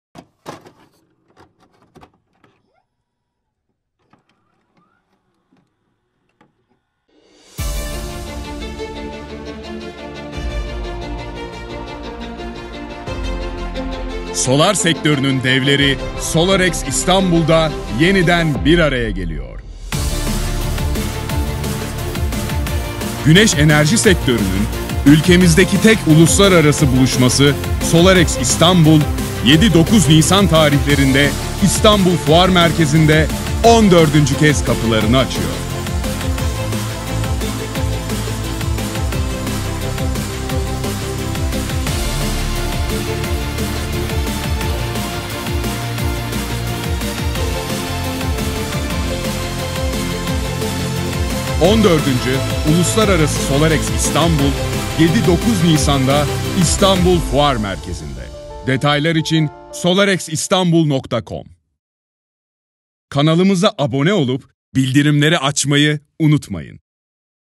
Türkiye'nin Tek Uluslararası Güneş Enerjisi Fuarı 7-9 Nisan'da İFM'de
Güneş Enerji Sektörünün Tüm Devleri Burada Buluşuyor. 14. Uluslararası Solarex İstanbul Fuarı 7-9 Nisan 2022'de İstanbul Fuar Merkezi'nde.

Ülkemizin ilk ve tek güneş enerjisi konulu fuarı olan Güneş Enerjisi ve Teknolojileri Fuarı “Solarex İstanbul”, Türkiye’nin güneş enerjisi alanında üretim merkezi olması yolunda büyük adımlar atmasına fırsat tanıyan bir fuardır.

Güneş enerjisi alanında dünyadaki son teknolojilerin ve Türkiye’de üretilen yeni ürünlerin bir arada sunulduğu ticari bir platform olan “ Uluslararası Güneş Enerjisi ve Teknolojileri Fuarı” sektörün lider firmalarının ve temsilcilerinin buluştuğu bir organizasyon olma özelliğine sahiptir.

Avrupa’nın en güneşli ülkelerinden biri olarak kabul edilen Türkiye’de bu konuya yoğunlaşan bir fuarın olması, dünya çapında tanınmış ve Türkiye’de yatırım yapmaya gönüllü çok sayıda dünya markası tarafından da yoğun bir ilgi ile karşılanmaktadır. Güneşlenme alanı oldukça yüksek olan ülkemiz, bu coğrafi özelliği avantaja dönüştürme konusunda en büyük desteği, “Solarex İstanbul” fuarı ile almaktadır.